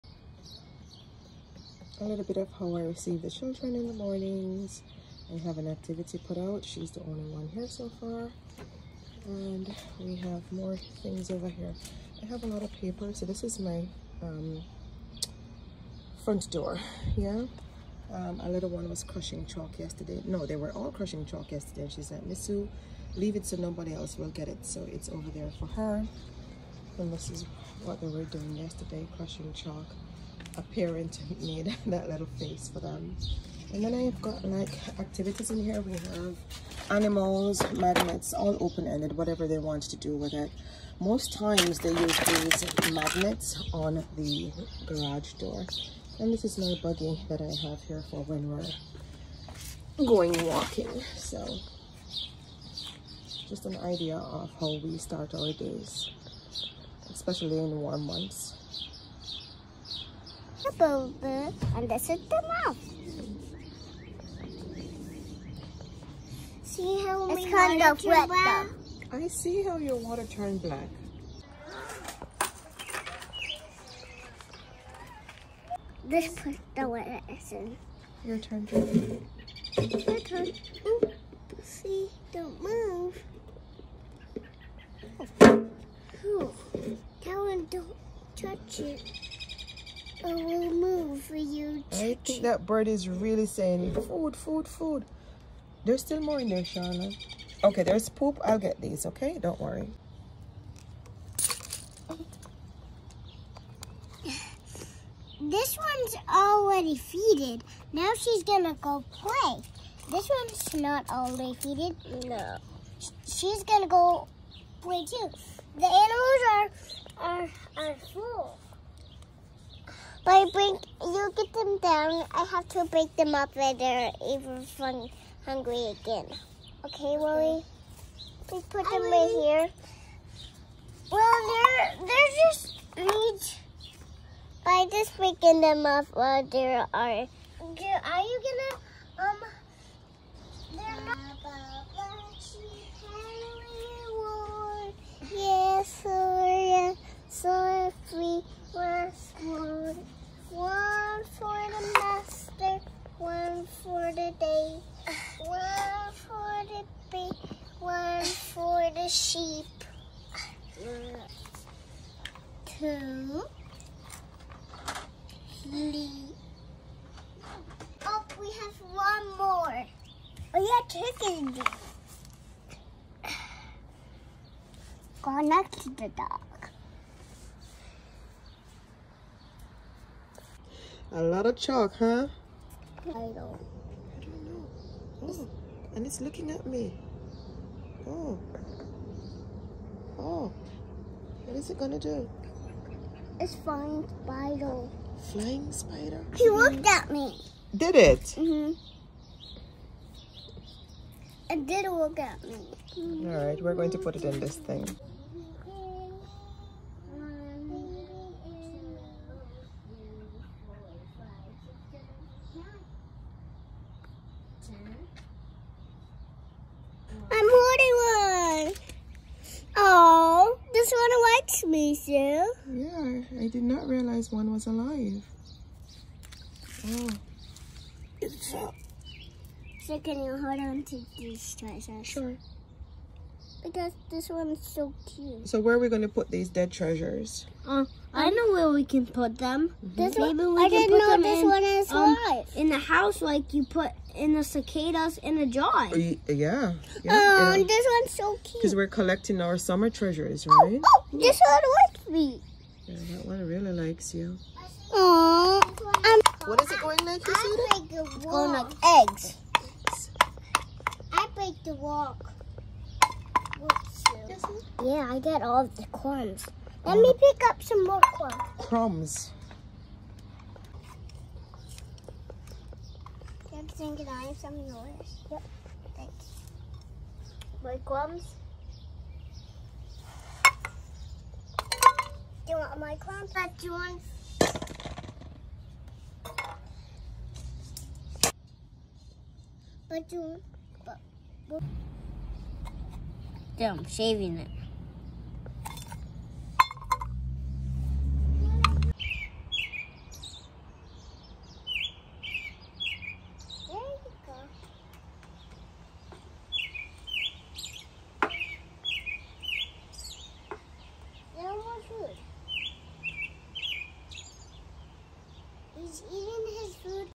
How I Receive The Children In The Mornings | Home Daycare Morning Routine
Hello, my fellow educators. Today we are looking at how I receive the childcare children in the mornings during the warm months.  We play outdoors for hours. The children arrive, and I  start the day at my patio / front door.  I have a caddy of toys for their play. Most mornings, we investigate with live animals from our garden or surroundings, we play on the black tarp, paint and build with magnets.  We head indoors for breakfast at 9:00am.  After breakfast, we have morning discussions and stories and head outdoors in our backyard.  We are in our backyard until it's time for a nap. The children eat lunch outdoors, which means I prepare lunch ahead of time, yes, before the daycare starts.  I also get help from my teenagers.
I hope you are having a wonderful summer so far.
xoxo Miss Sue